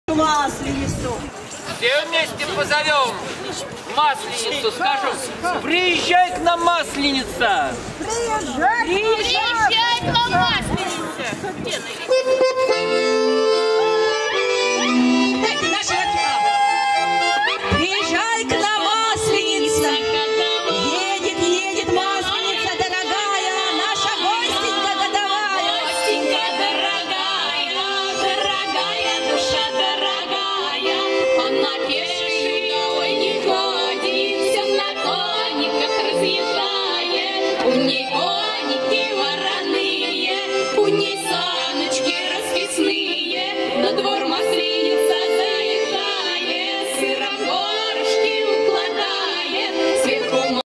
Масленичные гуляния организовали для жителей муниципального округа Даниловский
Специально к празднованию Масленицы аппарат Совета депутатов организовал мероприятие, которое состоялось на территории детского парка «Липки» в среду, 6 марта. Поздравить местных жителей прибыли руководитель аппарата Совета депутатов муниципального округа Даниловский Надежда Белова и депутат Ольга Подольская.